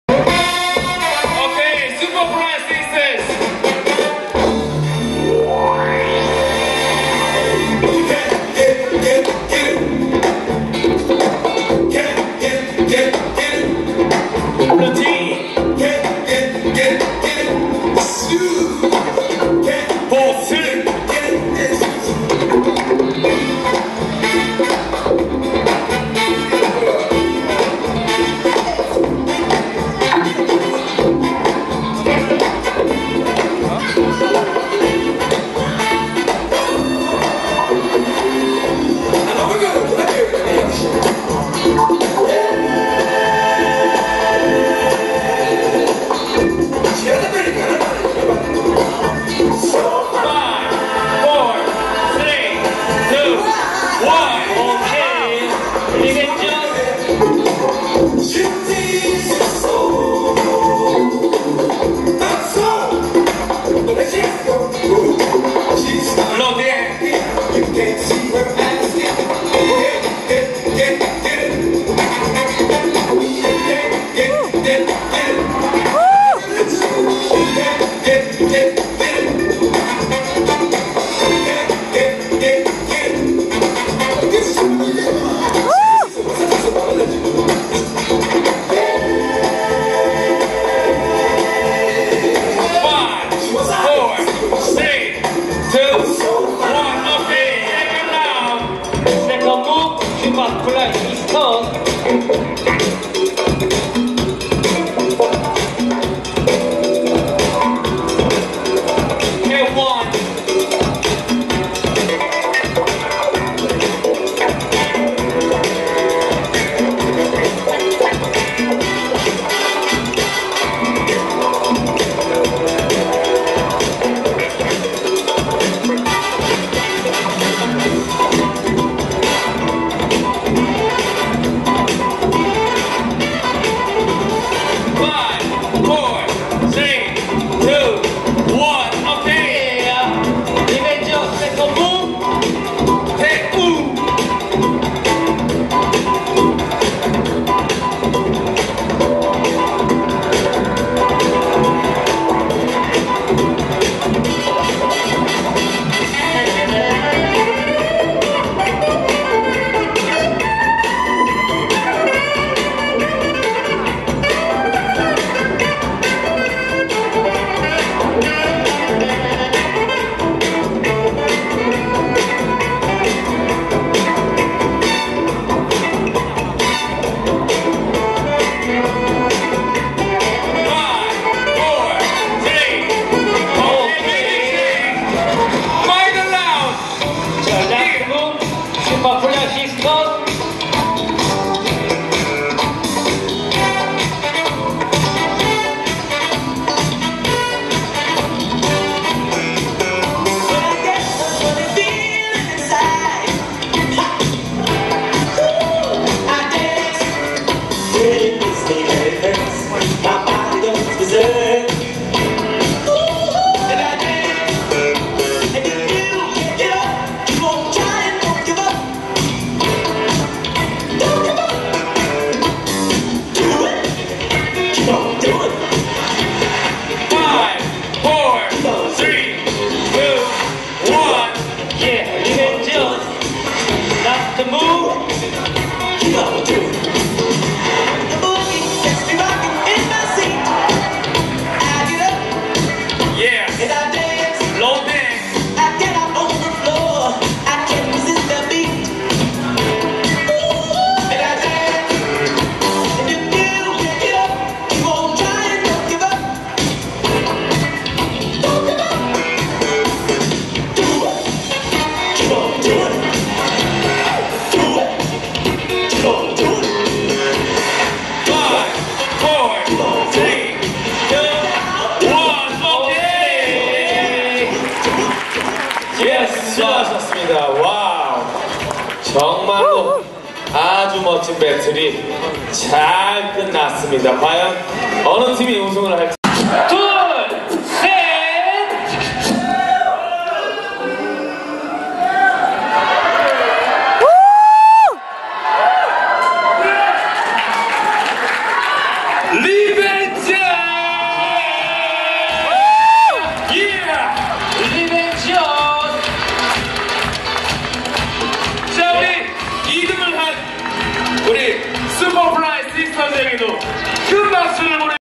(Locking) Final_ VS
(Locking) Final
슈파플라이시스터  VS 리벤져스

*Winner - 리벤져스

20111119 KEEP ON DANCING KOREA
@건국대학교 새천년홀